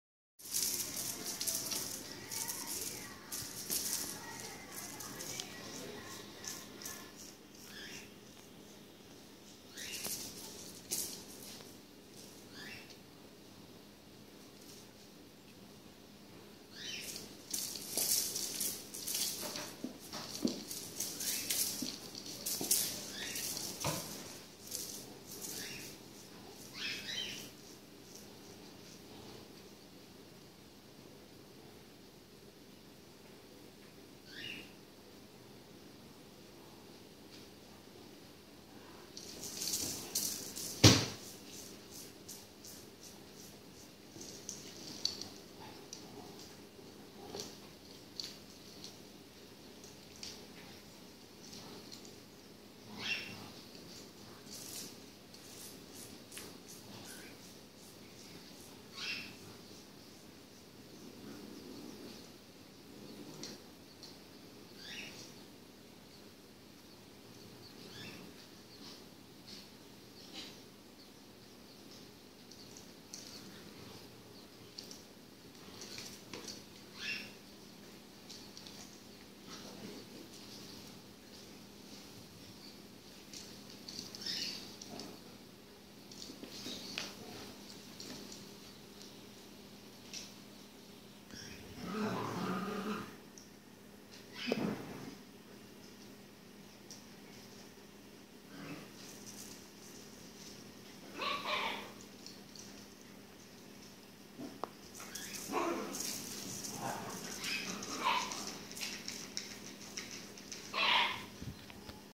GATO BRINCANDO
Vejam a gracinha que é a Pantera e a Kyra brincando quase o dia todo com seu novo brinquedinho.
São momentos incríveis da mais pura descontração para quem está por perto apreciando. Aguarde em breve mais vídeo de gatos engraçados e sempre brincando.